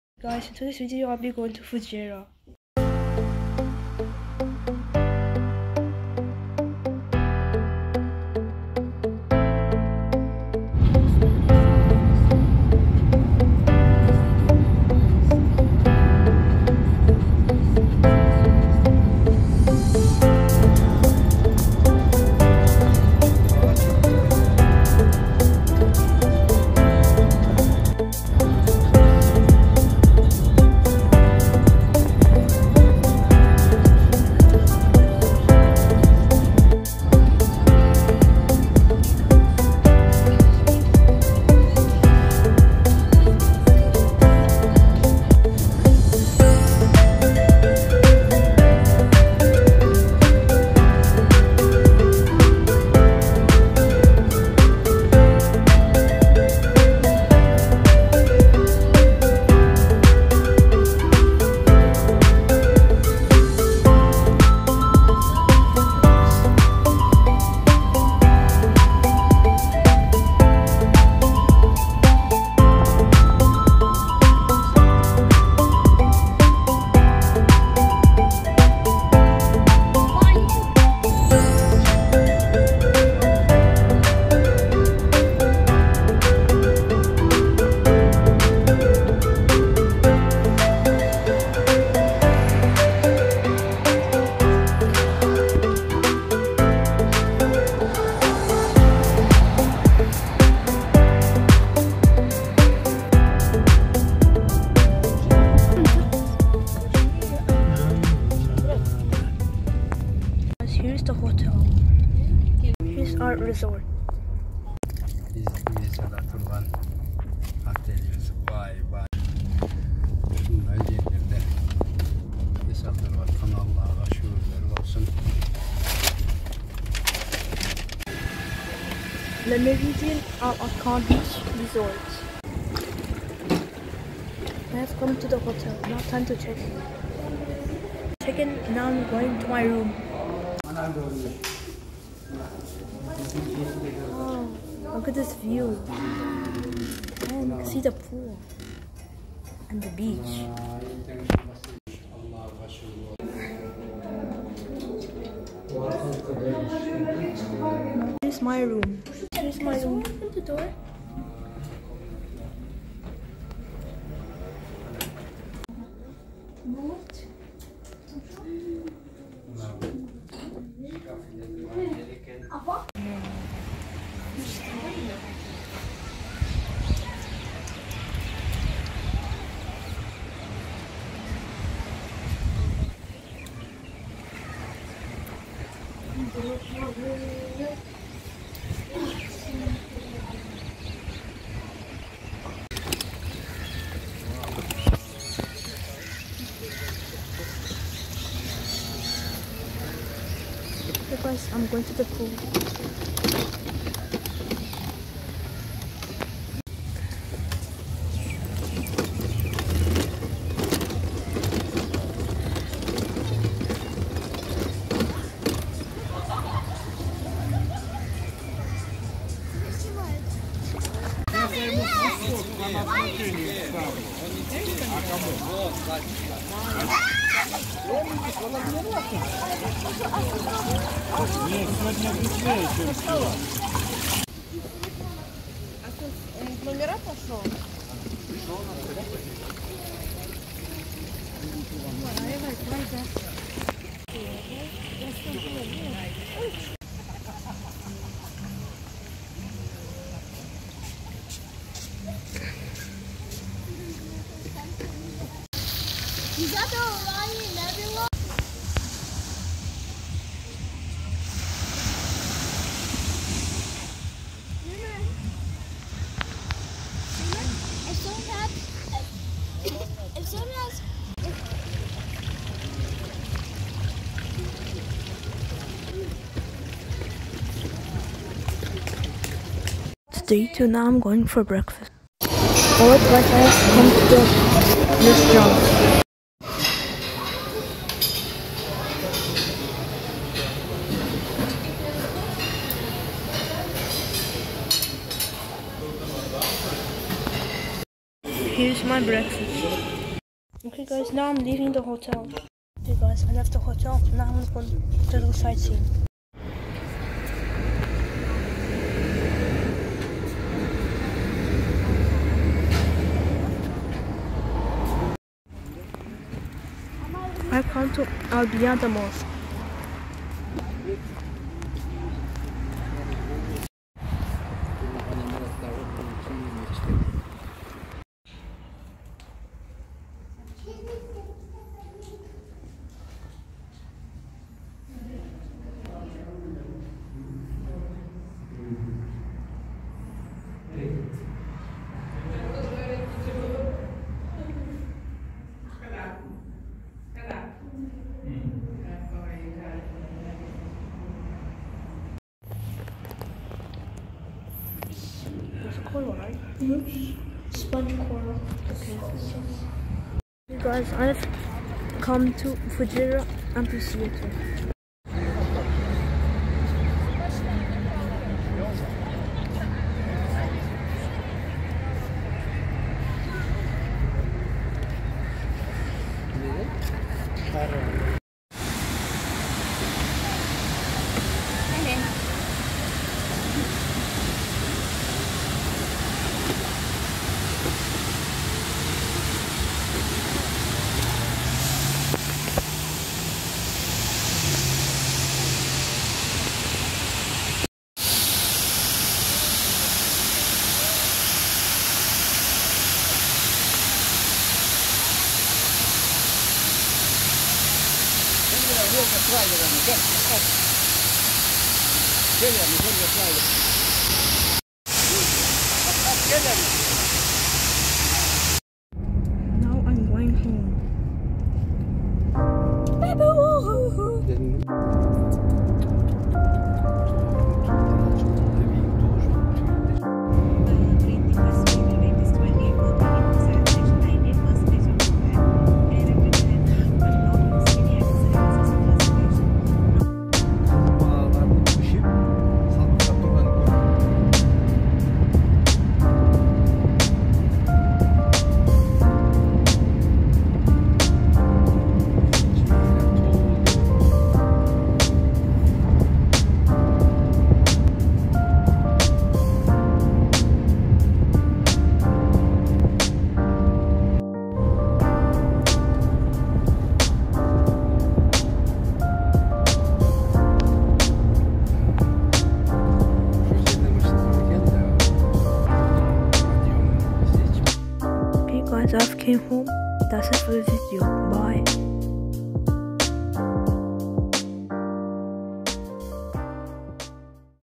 Fujairah travel vlog
In today's video, I will be going to Fujairah.